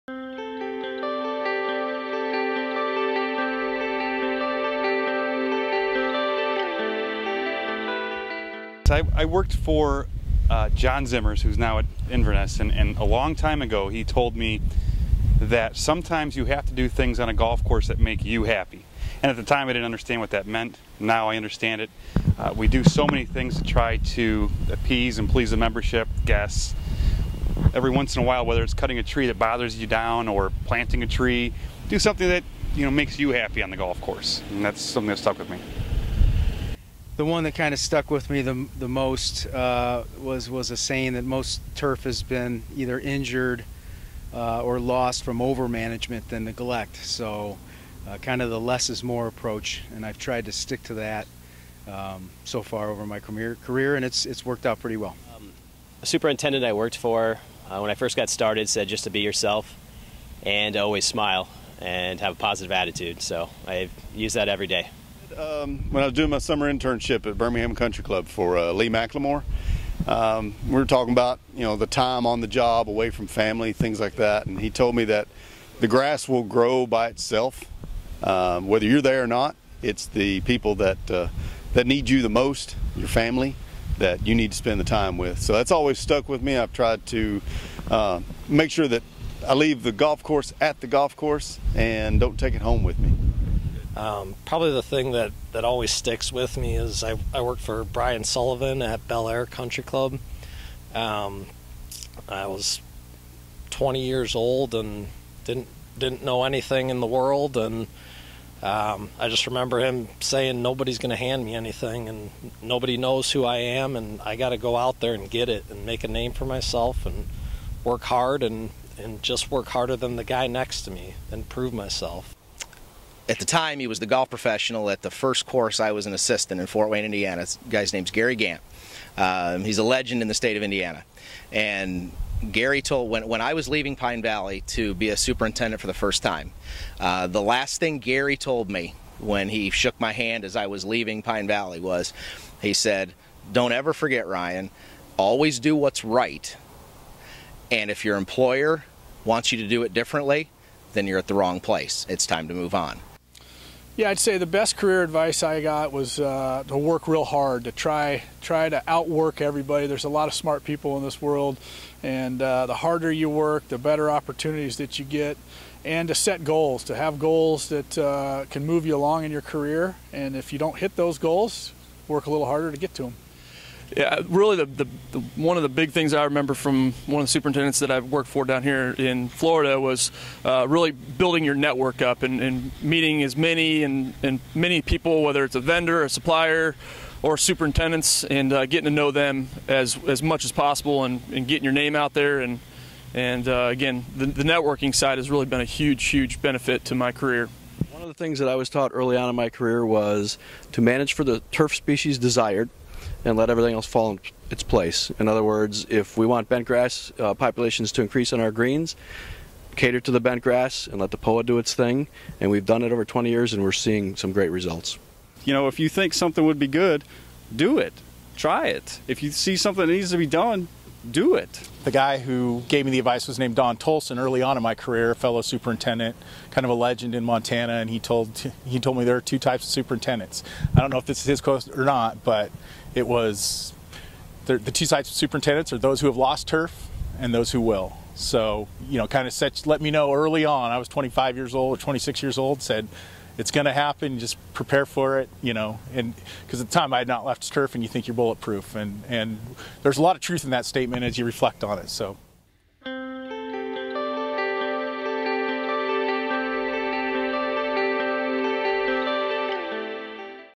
Career advice from superintendents across the country
Tune into this video to hear top superintendents share the advice that had a big impact on their career paths and helped them become successful in the golf course industry.